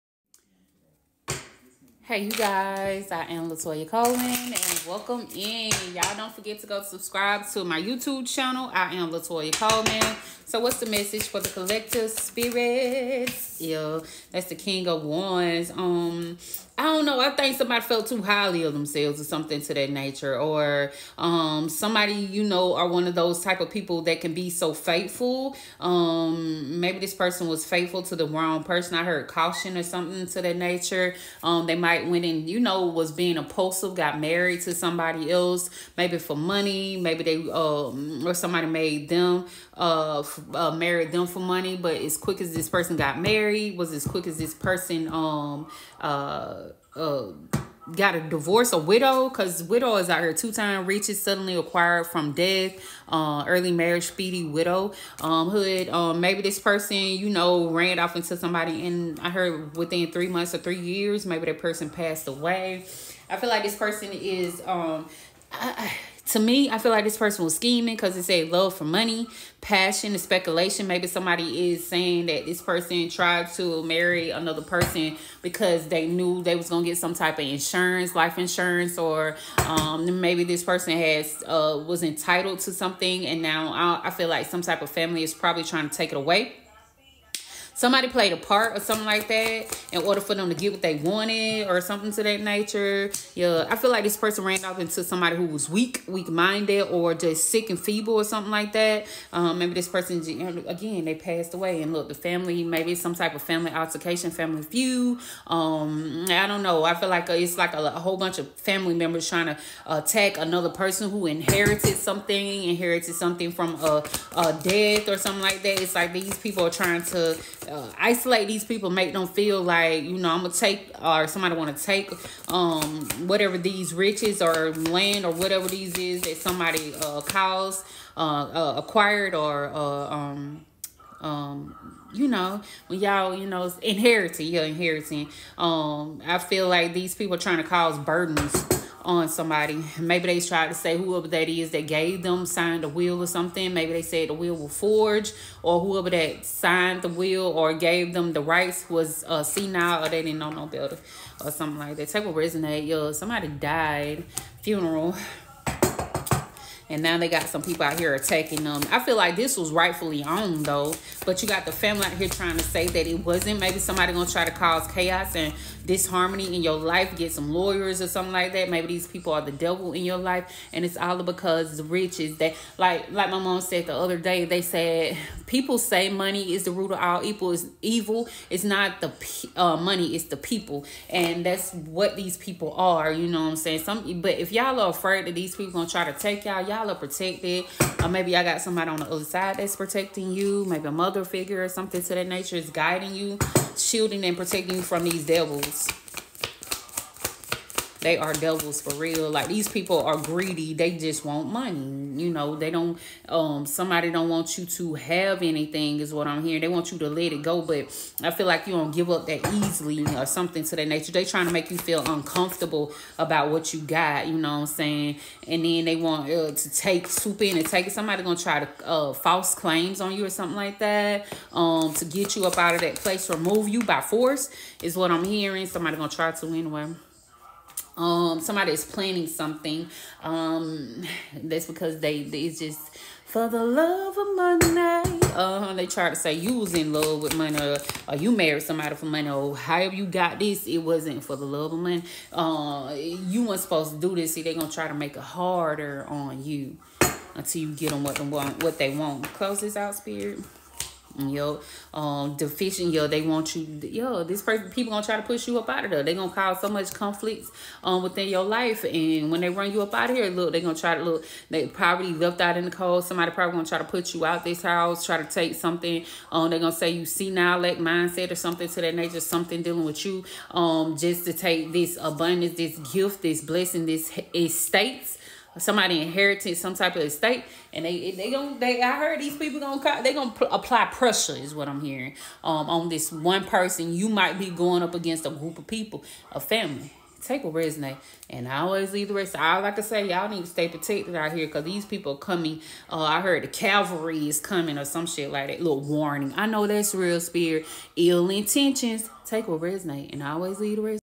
A family is funding over an inheritance! Be careful
Someone inherited an estate but the family is pissed that it’s you